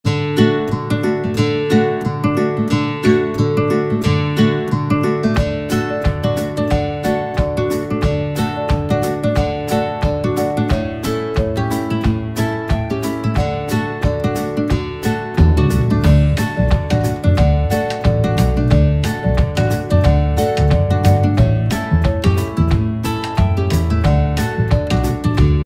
Pregnancy Goodie Bag for Birthing People - Stop Motion
In this video, I pack a pregnancy goodie bag for birthing people. I personally selected some of my favorite artworks to share with a friend giving birth!